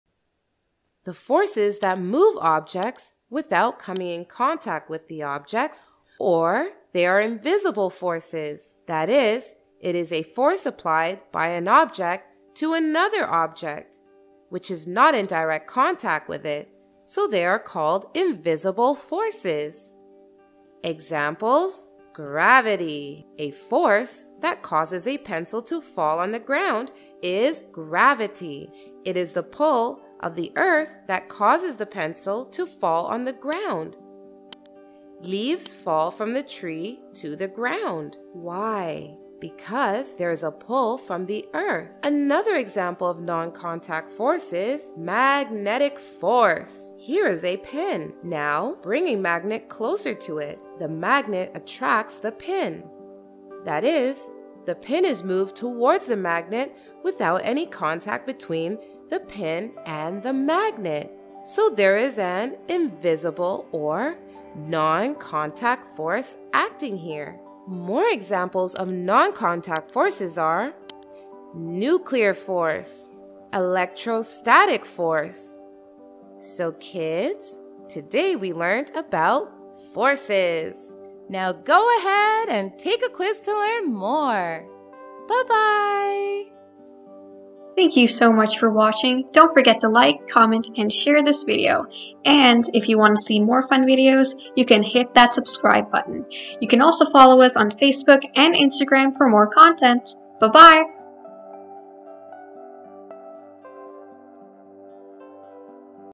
Forces | Non Contact Forces & Invisible Forces | Science
This Educational video explains about forces. This will help them a lot in their daily routine.
Forces
• Types of forces
• Non-contact forces/ Invisible forces:
o Gravity
o Magnetic force
o Electrostatic force
Forces | Contact & Non-Contact Forces | Direct Forces & Invisible Forces | Science